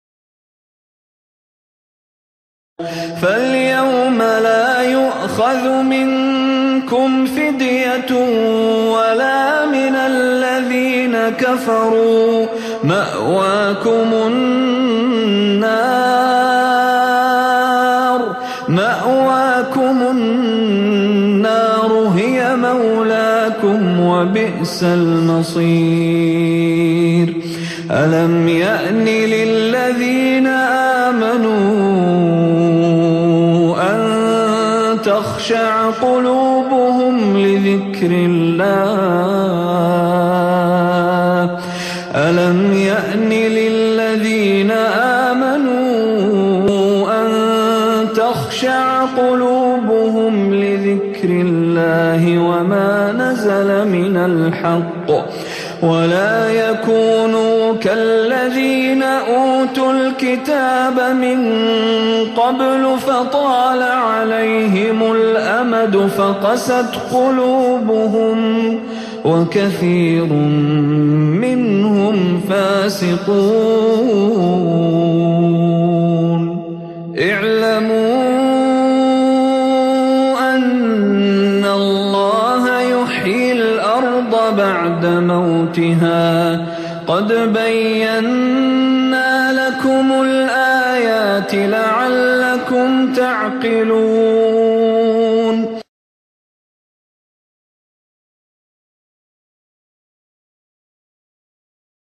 ( ألم يأن للذين ءامنوا أن تخشع قلوبهم لذكر الله )
سورة الحديد من الآية 15-17 برواية حفص عن عاصم بصوت الشيخ القارئ مشاري راشد العفاسي - حفظه الله -